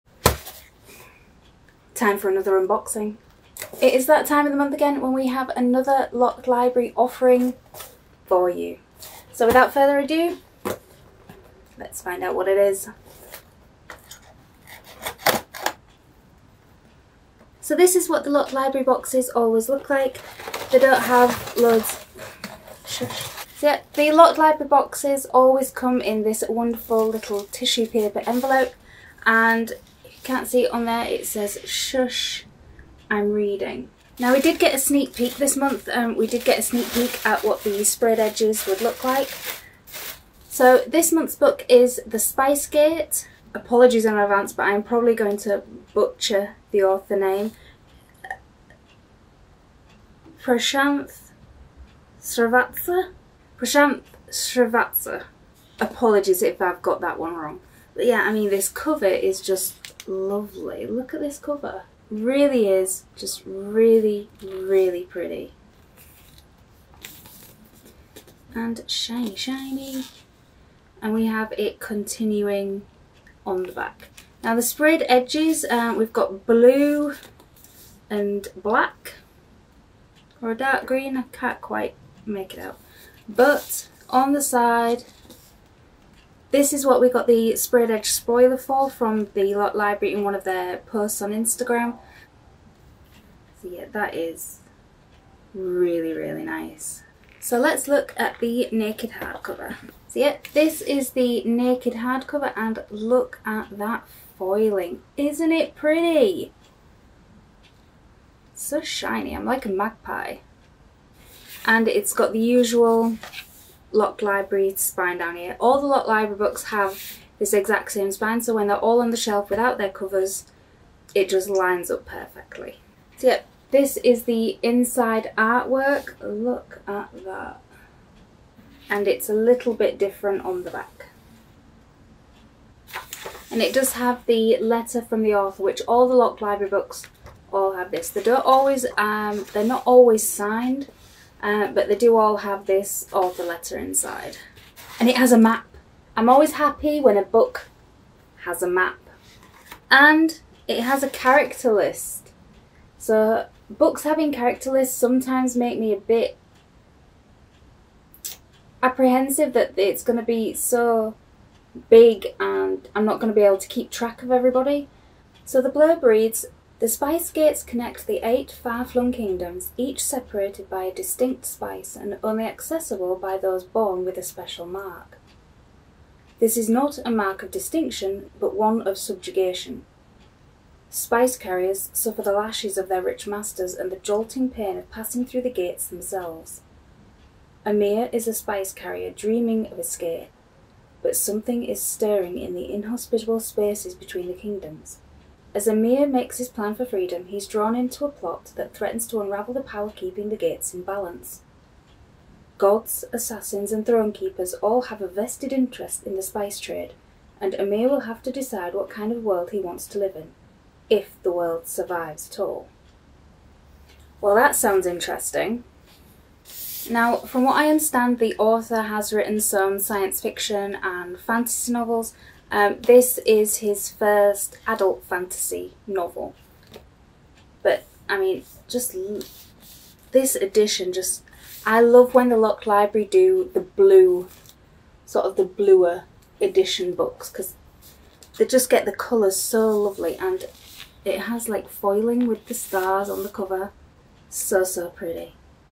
The Locked Library Unboxing - July 2024
Join me for unboxing the special edition book box for July!